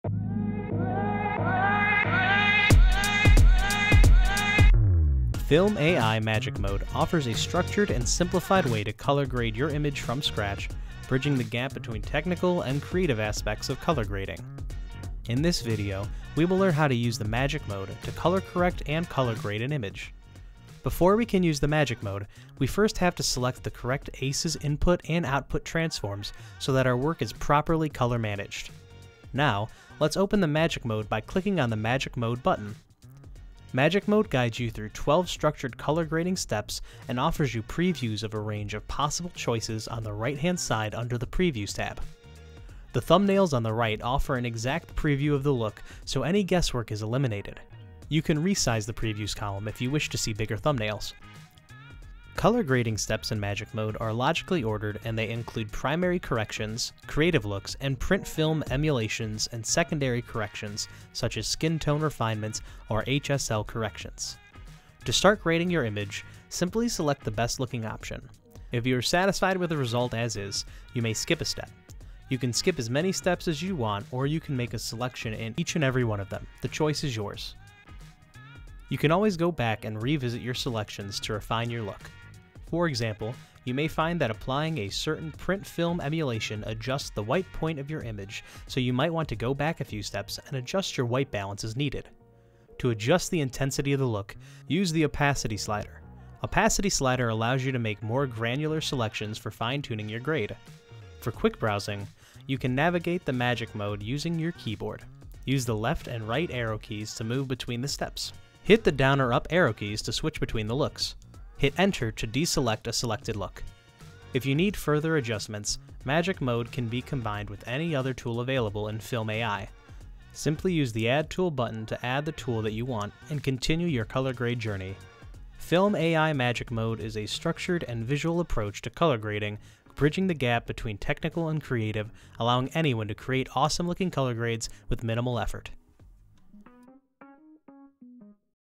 How to use the Magic Mode - a structured and simplified way to colour grading
fylm.ai is a revolutionary AI powered cloud-based colour grading platform allowing you to privately collaborate with your team anywhere on the planet

fylm.ai Magic Mode offers a structured and simplified way to colour grade your image from scratch with minimal effort, bridging the gap between technical and creative aspects of colour grading.

In this video we will learn how to use the Magic Mode to colour correct and colour grade your work.